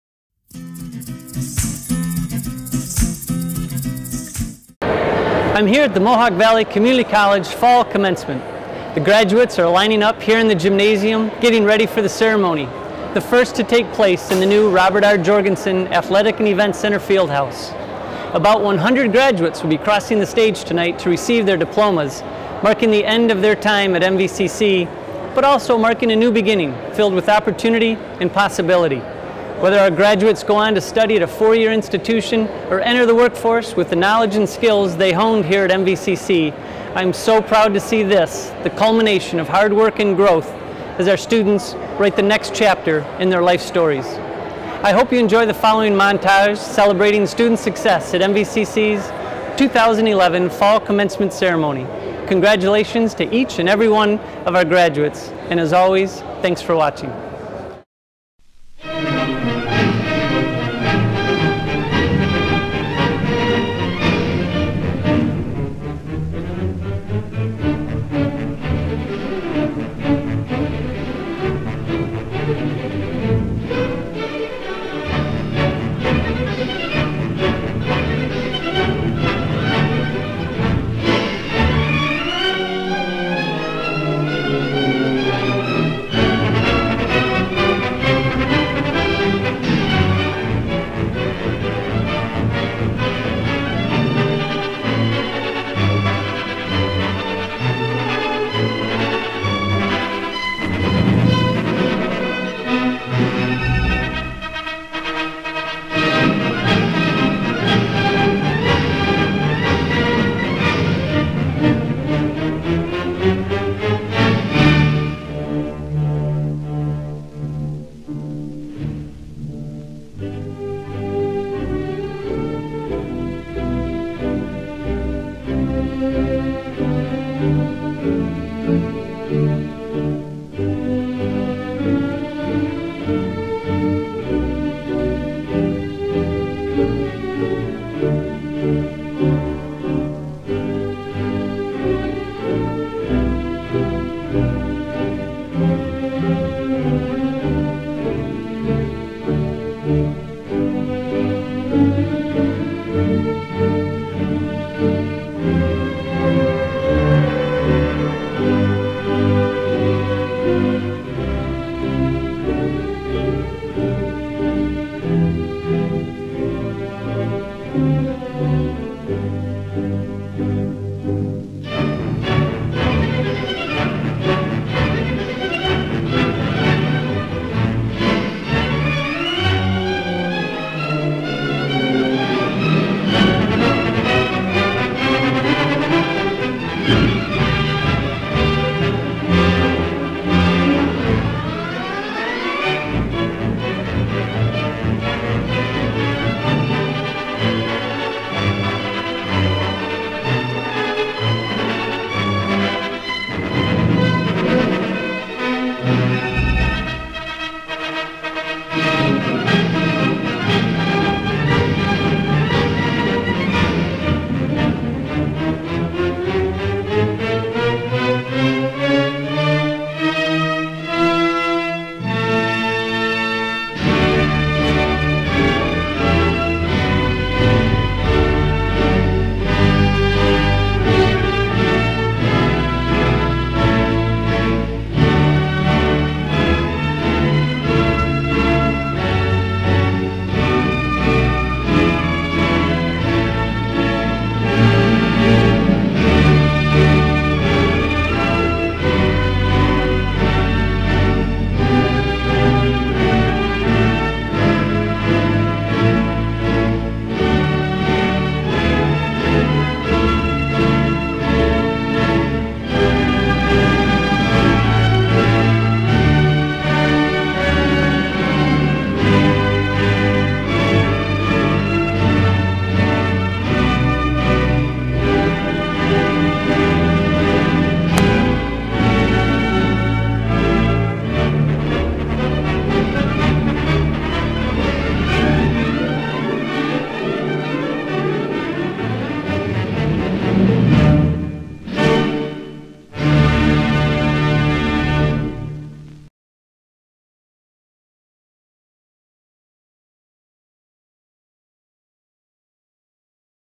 MVCC President's Blog: Commencement
Mohawk Valley Community College held its Fall Commencement ceremony on Thursday, Dec. 15, 2011, in the new Robert R. Jorgensen Athletic and Events Center Field House.